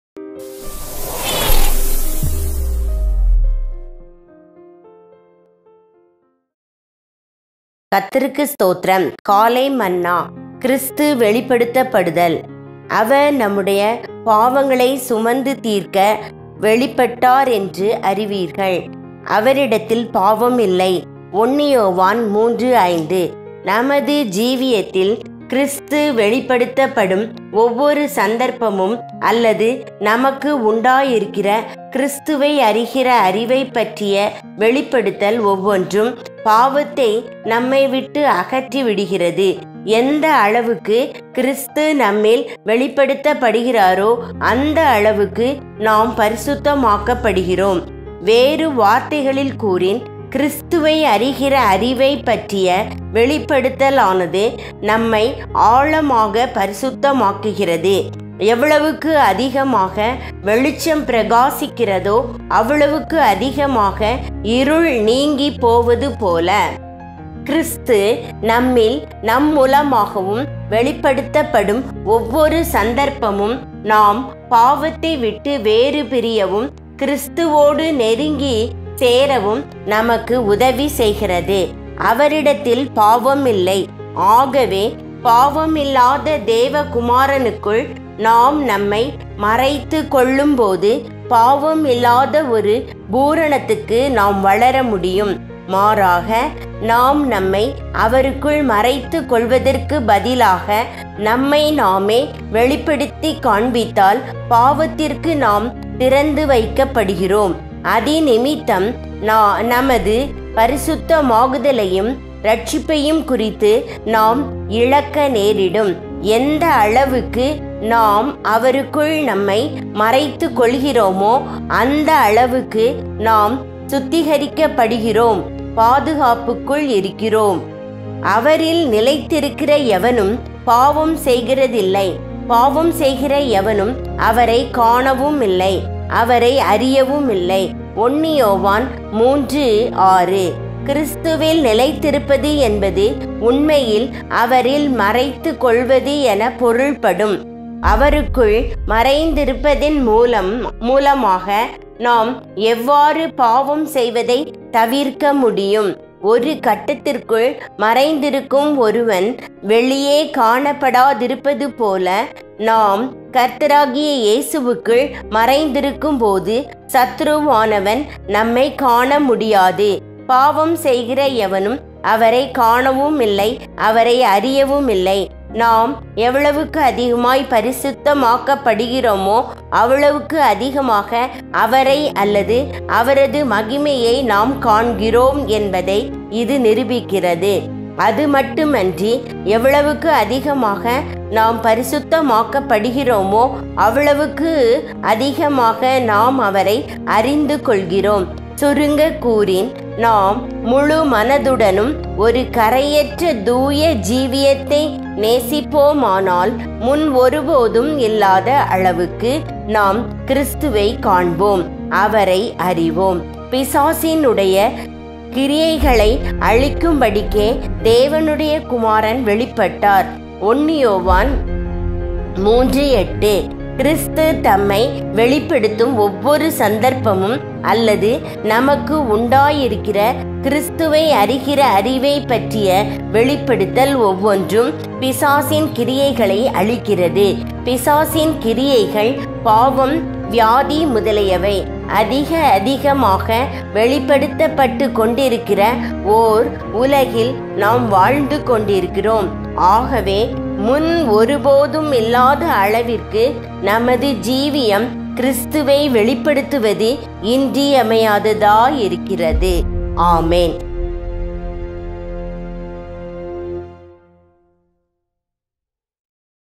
காலை மன்னா | MORNING MANNA | கிறிஸ்துவெளிப்படுத்தப்படுத்தல் | JUNE-30
JOHN_6:47,48
Verily, verily, I say unto you, He that believeth on me hath everlasting life.

I Am (JESUS CHRIST)The Bread Of Life.

யோவான்_6:47,48
என்னிடத்தில் விசுவாசமாயிருக்கிறவனுக்கு நித்தியஜீவன் உண்டென்று மெய்யாகவே மெய்யாகவே உங்களுக்குச் சொல்லுகிறேன்.

ஜீவ அப்பம் நானே(கர்த்தராகிய இயேசு கிறிஸ்து ).